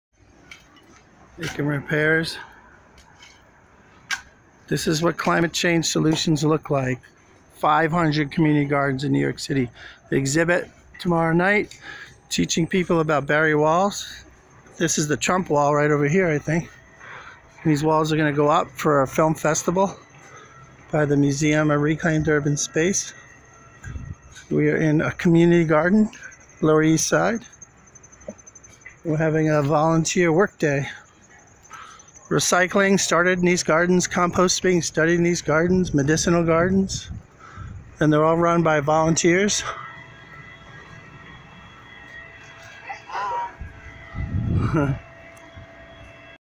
Volunteer Garden workday
Climate change solutions happening everyday working with time's up environmental organization in New York City New York City
NYC has500+ community gardens teaching people environmentally sustainable way of life